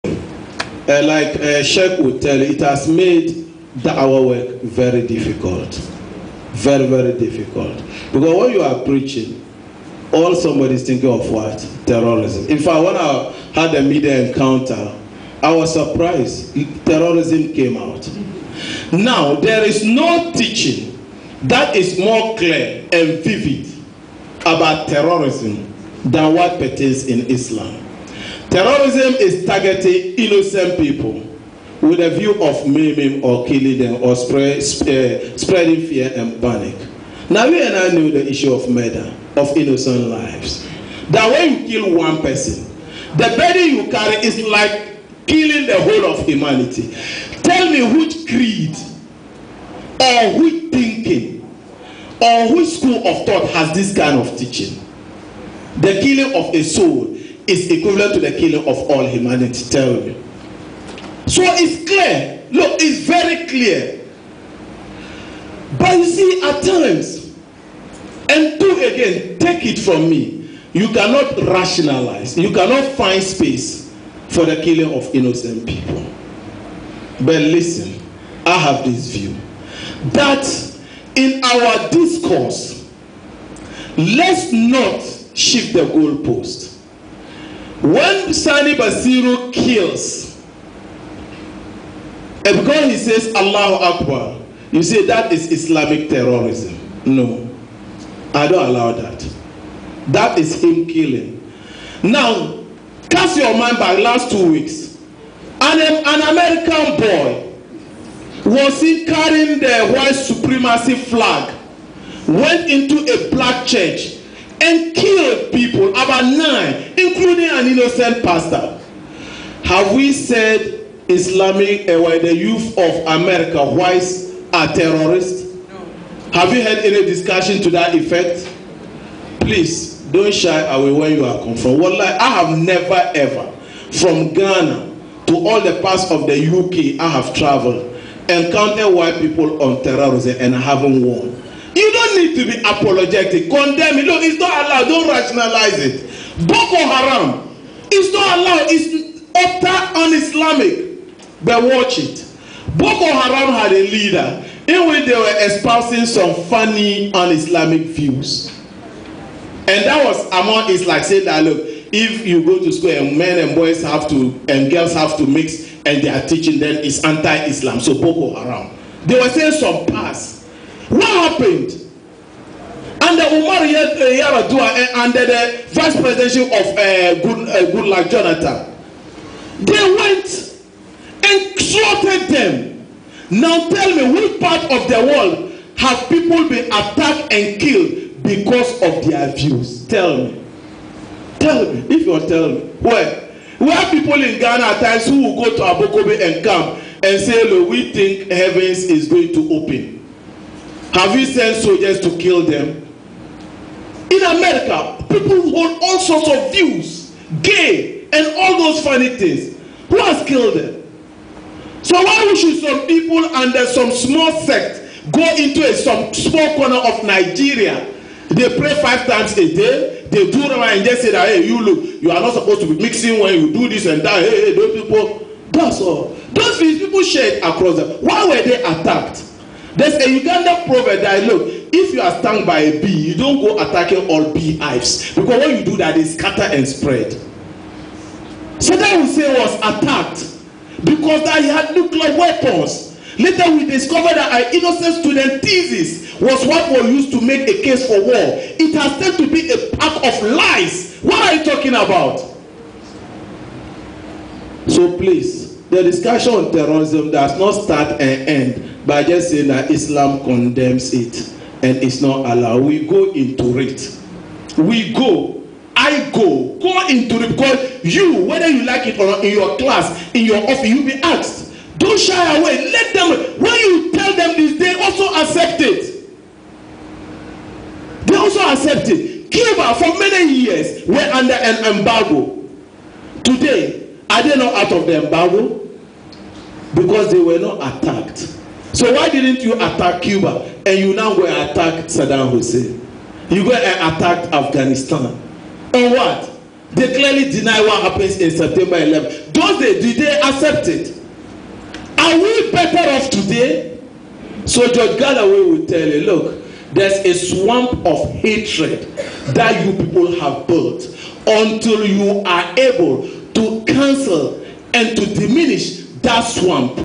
Islam and Terrorism By Hajj Dr Bashiru Shani
Hajj Bashiru Shani is a former head of PRO the Ghana Muslim Academy, he is a lecturer at the Accra Polytechnic, Board Member of the Internal Auditing Agency, Office of the President, Ghana.